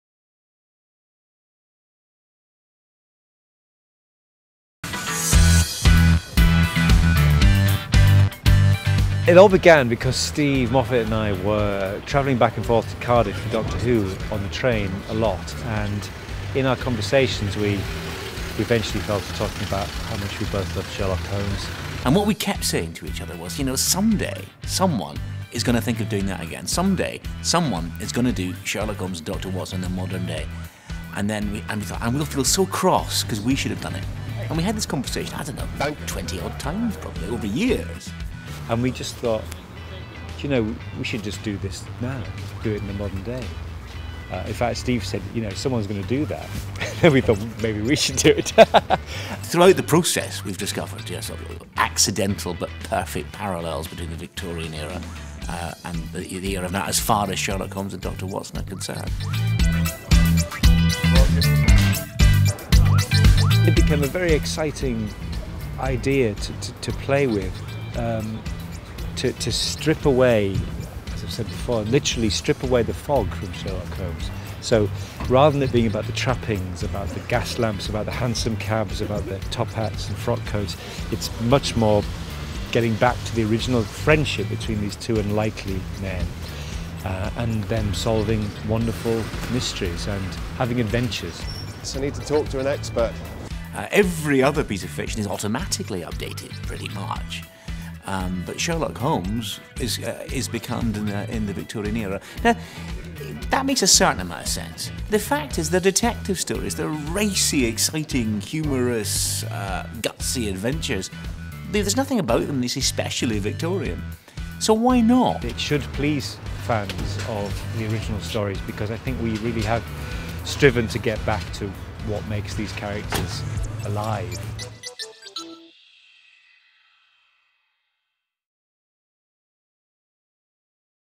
Steven Moffat and Mark Gatiss interview - Sherlock - BBC One
Co-creators and writers, Steven Moffat and Mark Gatiss talk about creating a modern Sherlock while on set with Benedict Cumberbatch and Martin Freeman.